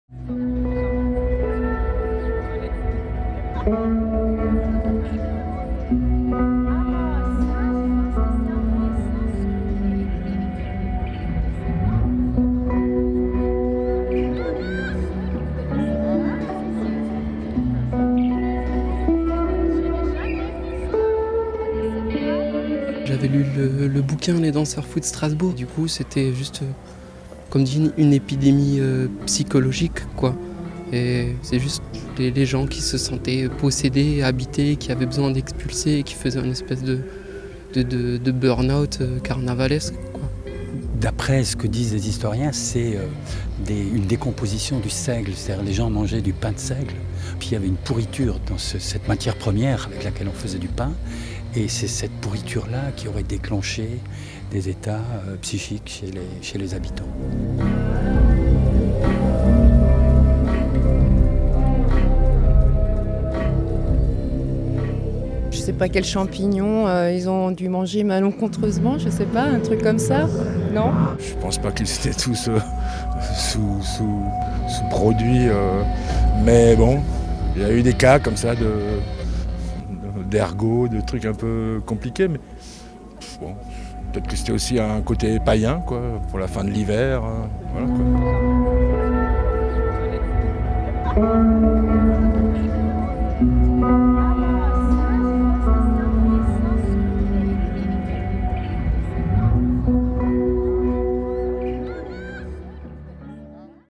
2018 : une nouvelle épidémie de danse à Strasbourg
500 ans après la première épidémie de danse qui frappa Strasbourg, la frénésie s'empare de la place du château au pied de la Cathédrale Notre Dame de Strasbourg.

Le 15 juillet 1518, une singulière épidémie de danse collective s’empare des corps à Strasbourg. Elle durera des semaines. Ce phénomène étrange ébranle la communauté strasbourgeoise. Le 500ème anniversaire de cet événement fait l’objet d’une exposition « 1518, la fièvre de la danse » au Musée de l'Oeuvre Notre-Dame de Strasbourg (officiel)